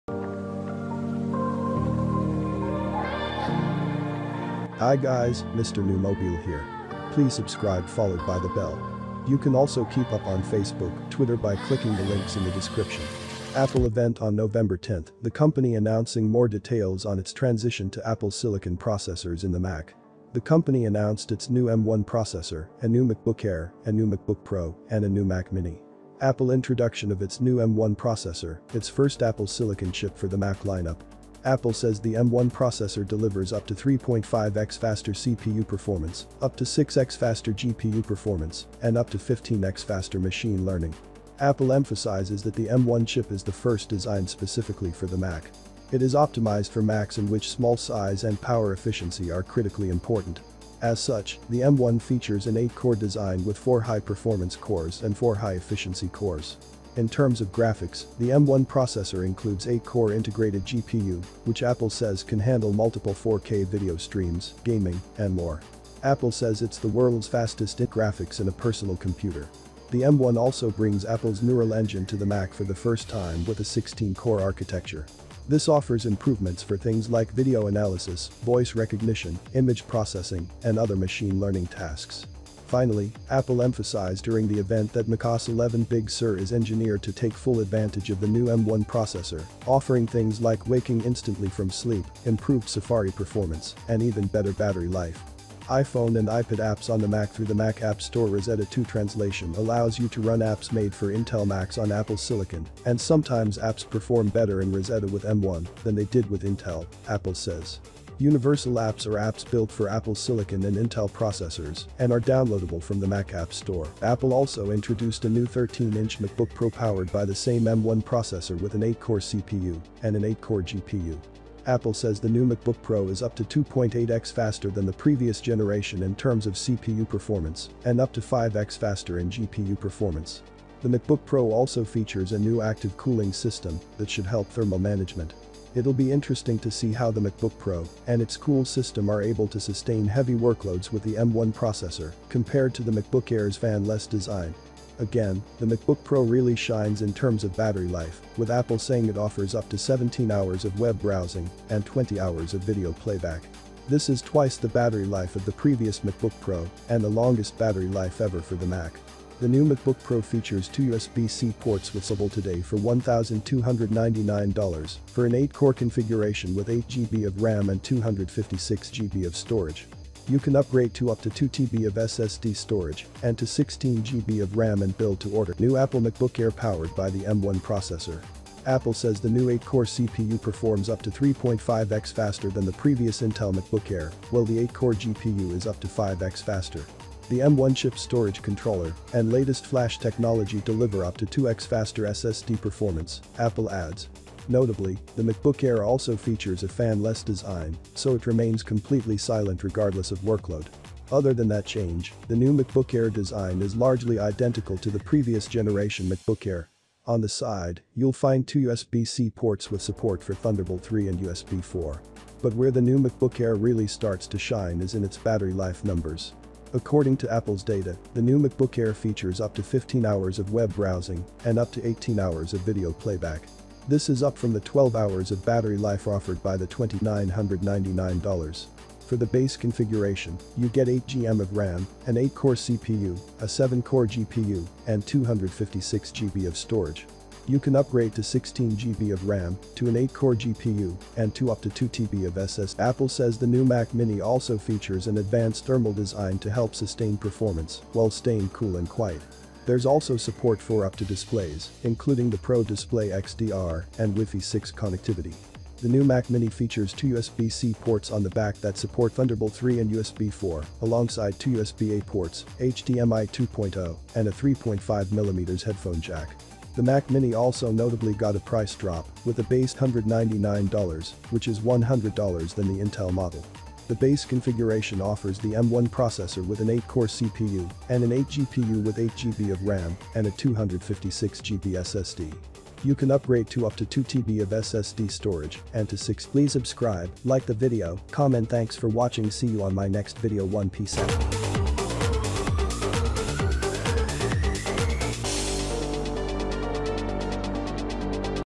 Apple M1 Chip, 13 inch MacBook Pro, New MacBook Air, Apple Mac mini, and More!!
Apple announced Three brand new Macs today powered by its new M1, a 13-inch MacBook Air, a 13-inch MacBook Pro, and a new Mac mini.